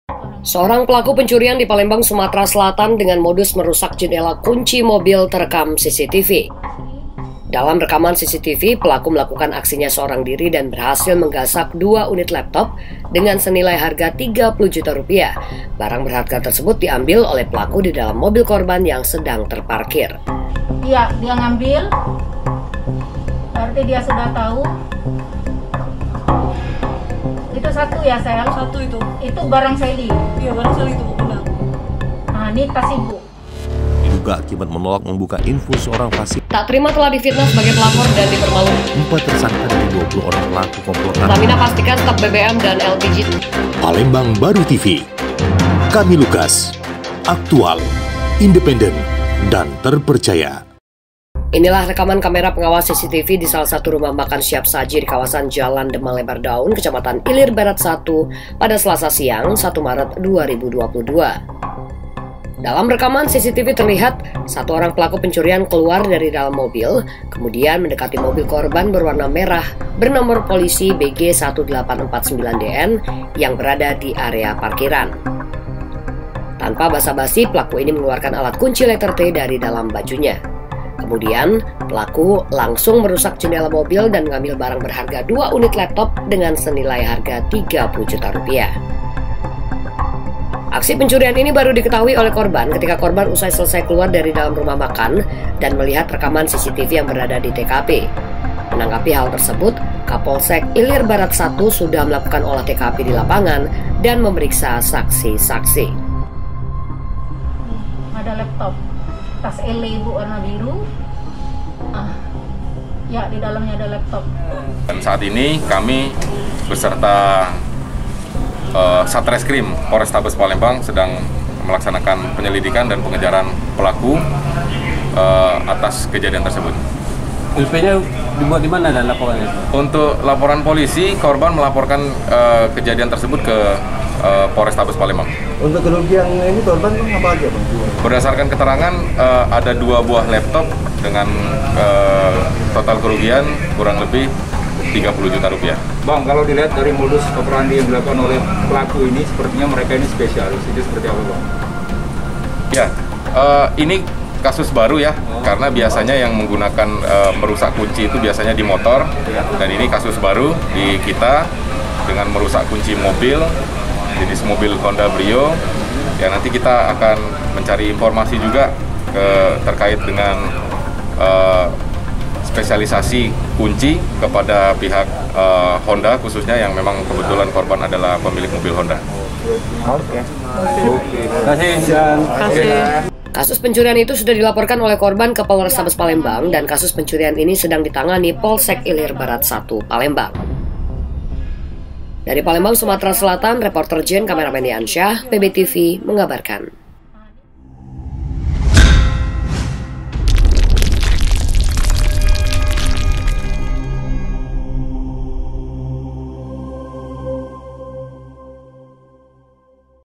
Aksi Pencurian di Parkiran Rumah Makan Sederhana Terekam CCTV, Polisi Buru Pelaku
Reporter : Jane
Kameramen  : Apriyansyah